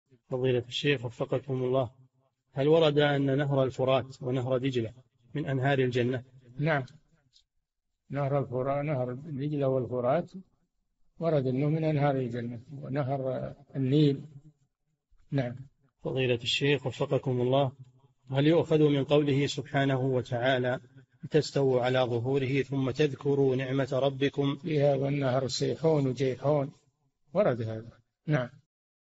[4613- 4717] هل ورد أن نهر الفرات ودجلة من أنهار الجنة؟ - الشيخ صالح الفوزان
"[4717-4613] هل ورد أن نهر الفرات ونهر دجلة من أنهار الجنة؟
الجَوَابُ: نعم، نهر دجلة والفرات ورد أنهم من أنهار الجنة، ونهر النيل، ونهر سيحون وجيحون، ورد هذا.

للاشتراك في قناة الشيخ صالح الفوزان
فتاوى المجالس في تفسير المفصل

للاستفسار والإقتراحات والدعم
رئيس الفريق
لمشروع كبار العلماء